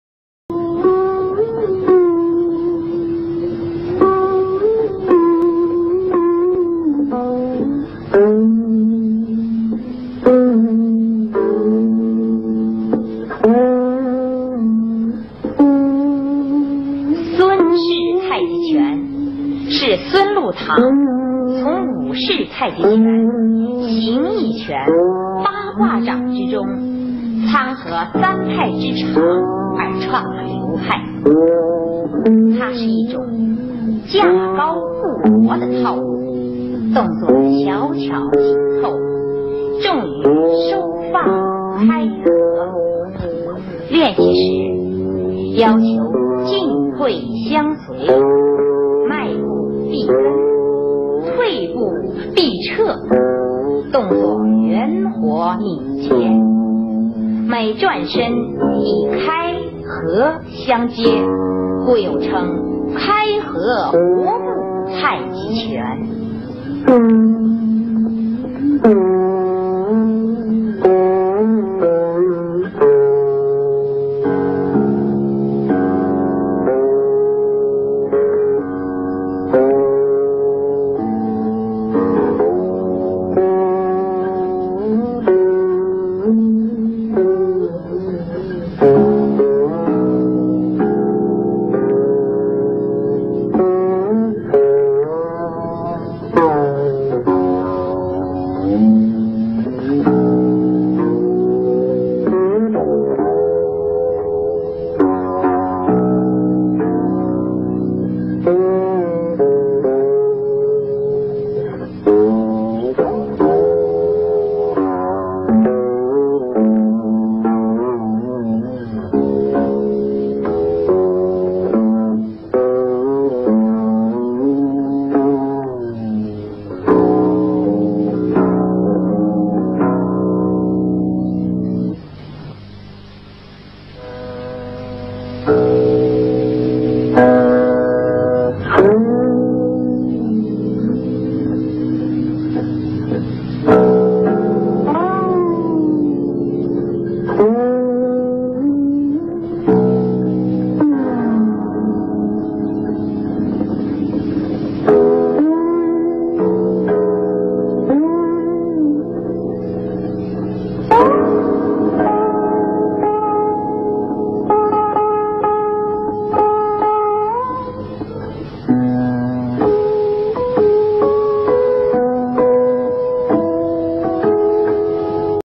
Sun Jianyun - 1984 Masters Demonstration
Sun Jianyun performs the Traditional Sun style of Tai Chi Chuan in a Tai Chi masters demonstration in 1984.